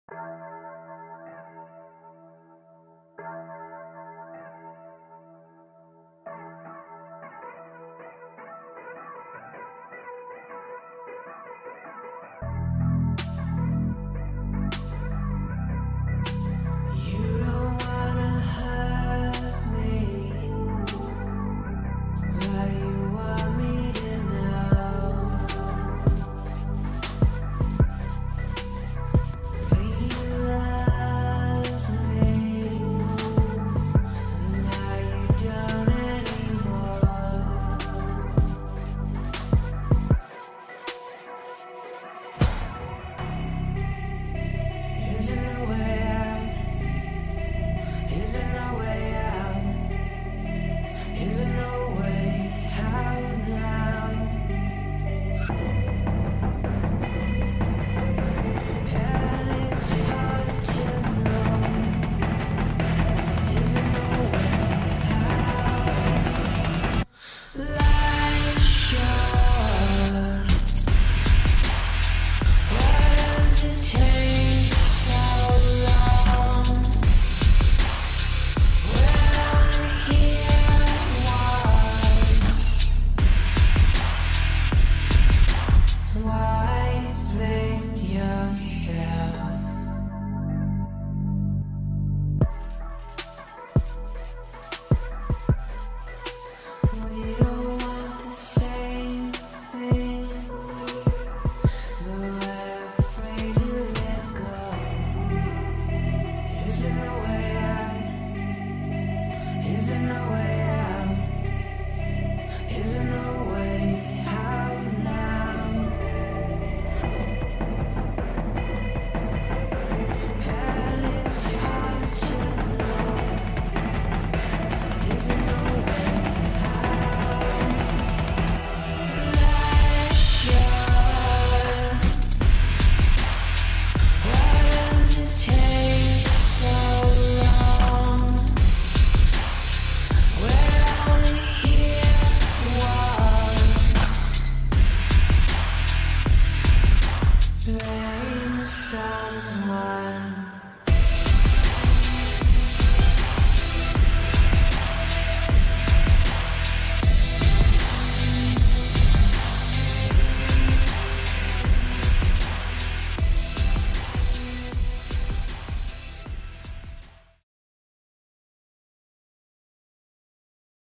(Blender addon)Flower growth free version：Phalaenopsis
Thank you for  purchase during this period. now open sourcing a free butterfly orchid flower plugin, which, like my other flowers, allows for free baking of the grid, natural collision between petals, growth effects, and high-quality textures. I hope you will like it and let Blender make flowers like Houdini~